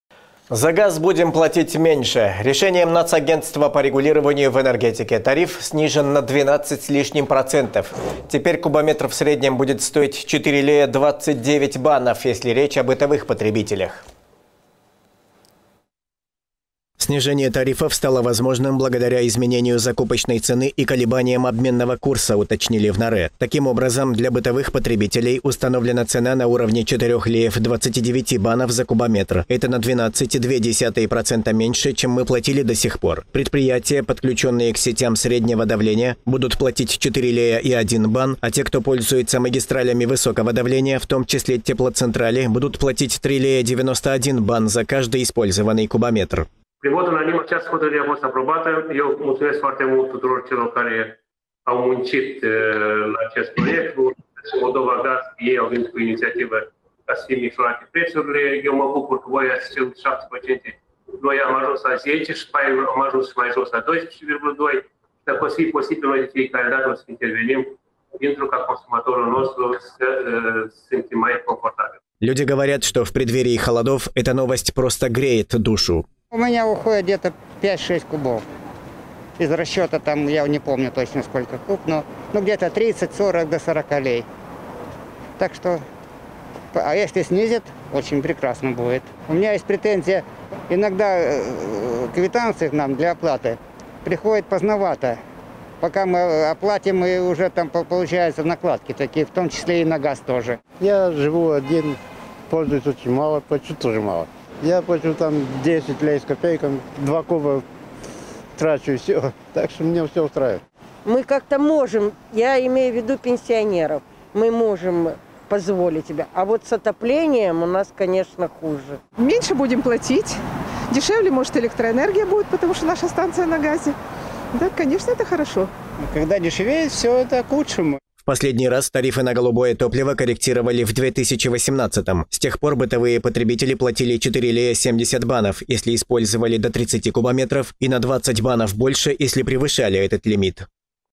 ТАРИФЫ НА ГАЗ СНИЖЕНЫ!
За газ будем платить меньше. Решением Нацагентства по регулированию в энергетике тариф снижен на 12 с лишним процентов - теперь кубометр в среднем будет стоить 4 лея 29 банов, если речь о бытовых потребителях.